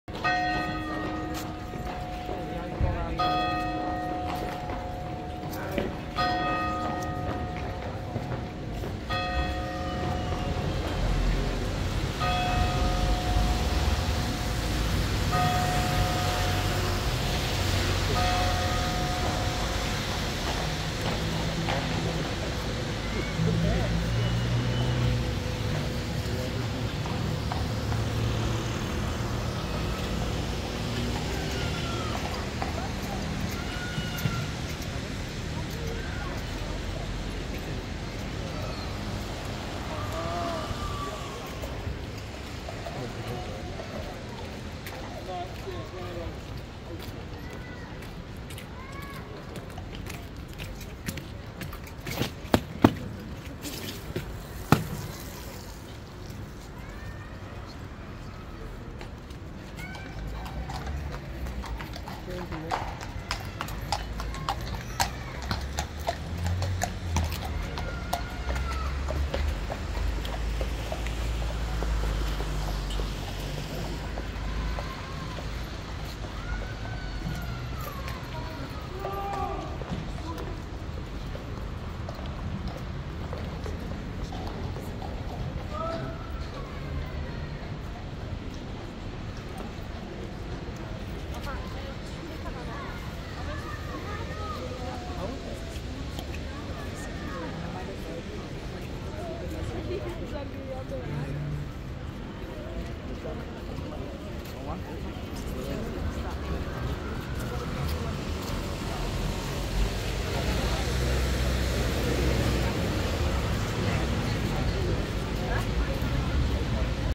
The Horse Was Stressed in the Changing of the Guards at Horse Guards | London, England
Sunday 03/12/2023 at Horse Guards in London